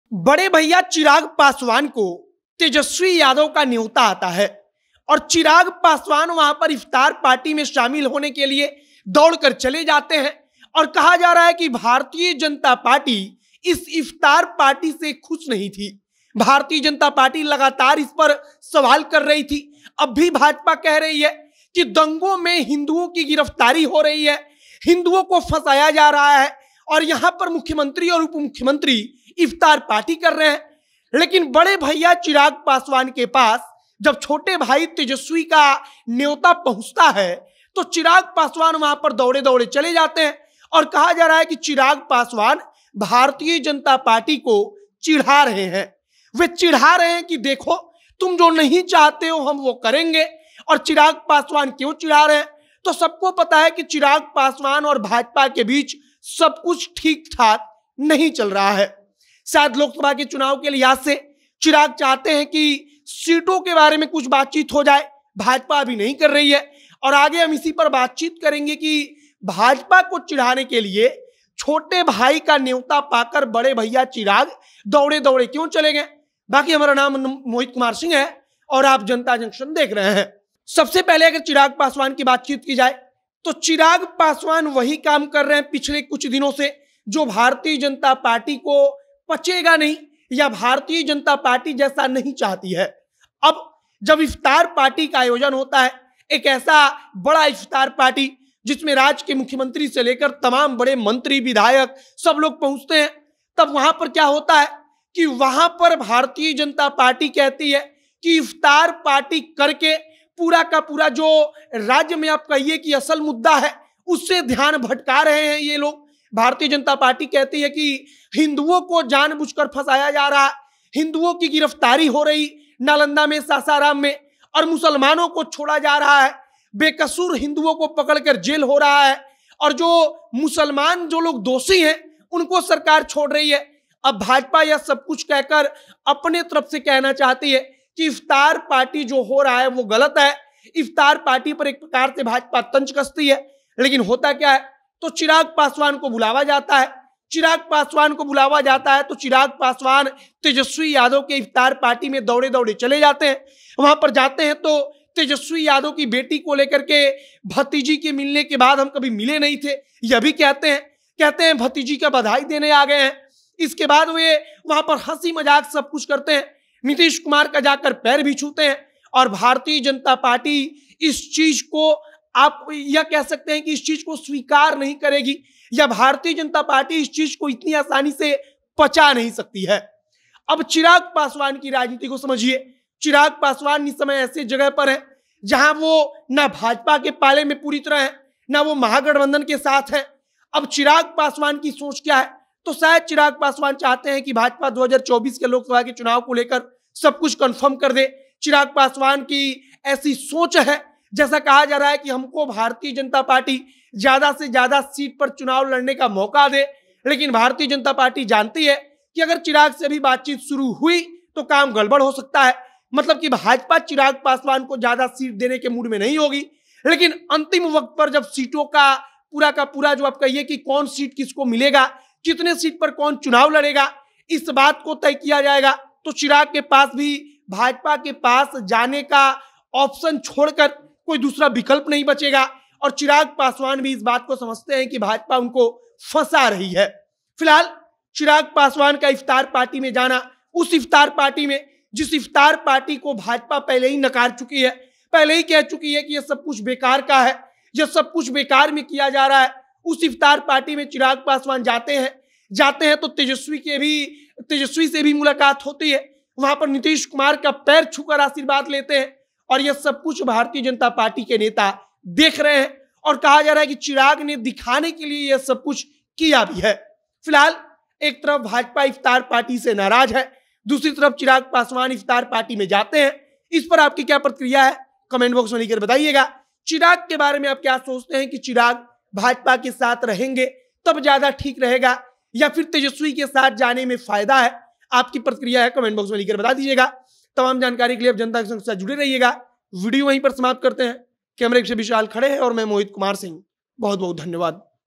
Chirag का Tejashwi के घर जाना BJP को चिढाना है! समझिए कैसे BJP को रास्ते पर लाने की कोशिश है!
छोटे भाई तेजस्वी यादव का बुलावा आना पर बड़े भईया चिराग का दौड़ते हुए चले जाना, भाजपा को चिढ़ाने के जैसा है! भाजपा इफ्तार का विरोध करती रही लेकिन फिर भी चिराग गए!